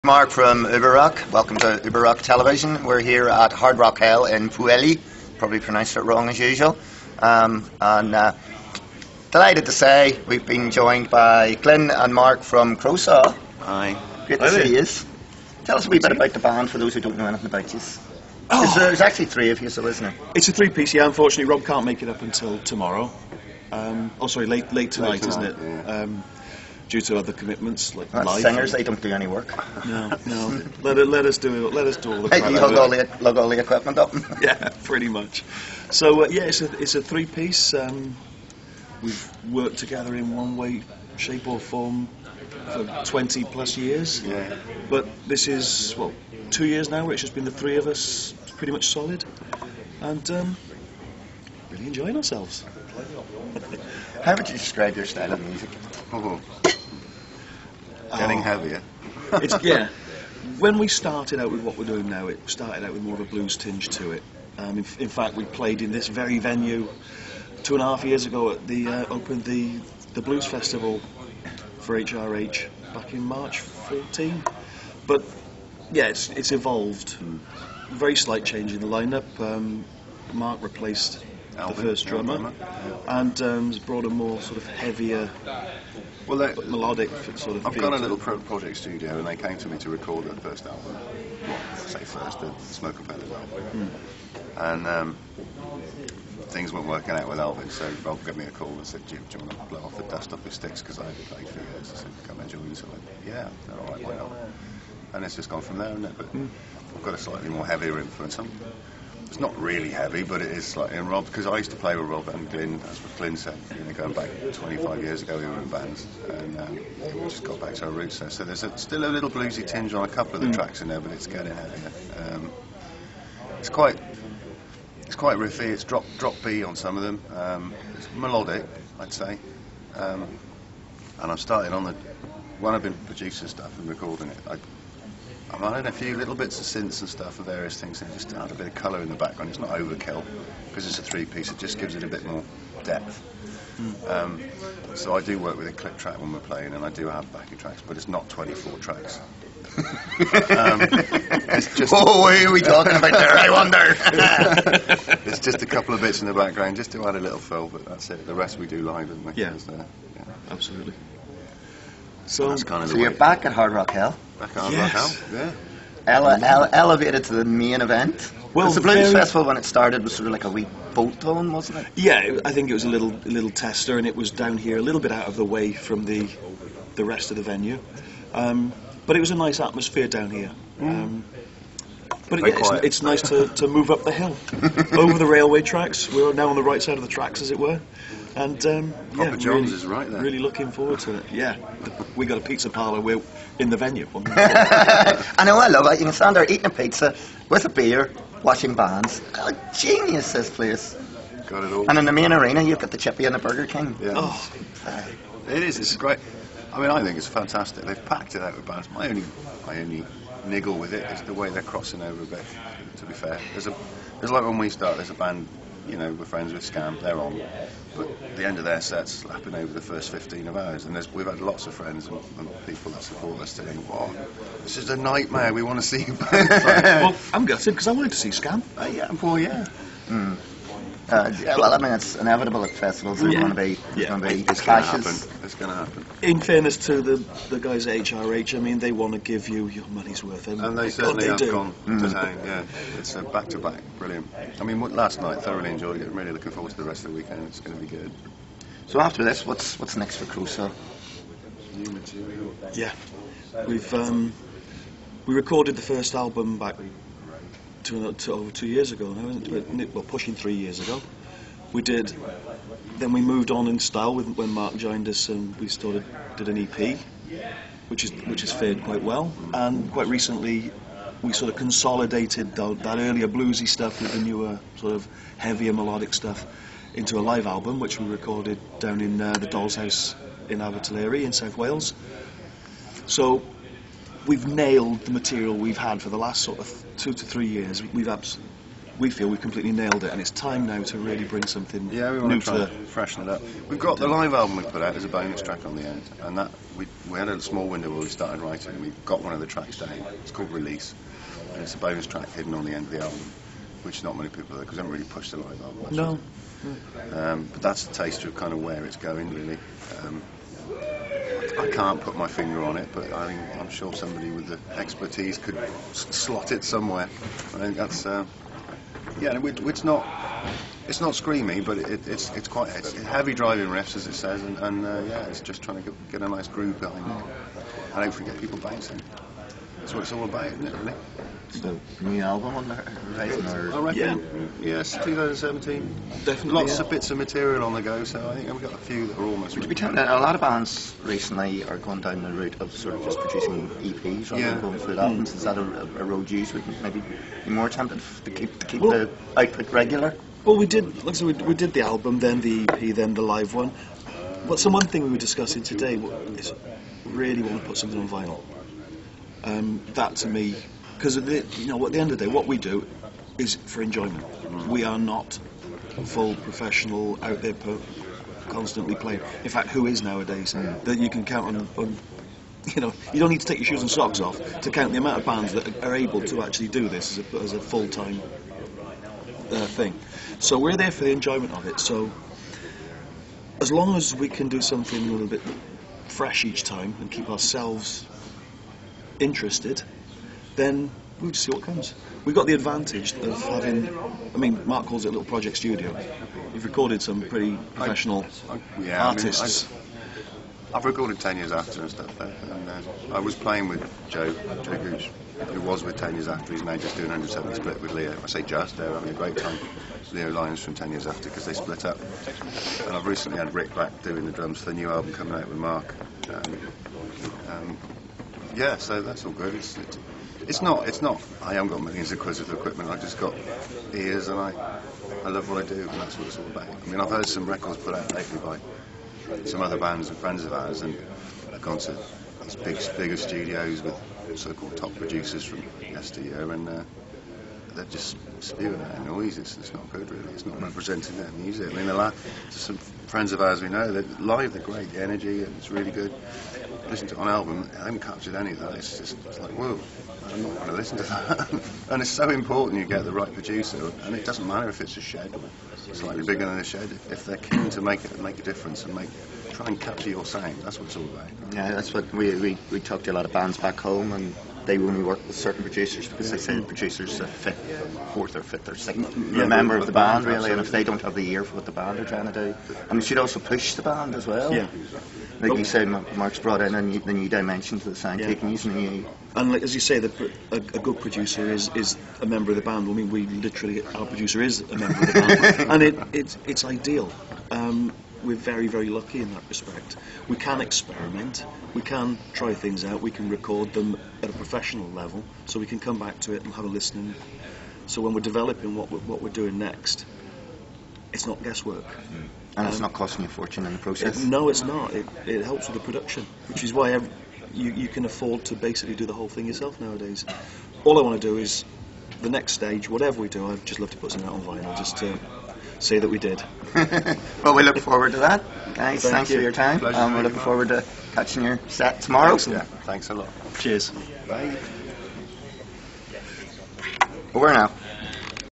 Uber Rock interview with Crowsaw Hard Rock Hell, November 2016
Uber Rock interviews Glyn and Mark from Crowsaw at Hard Rock Hell X.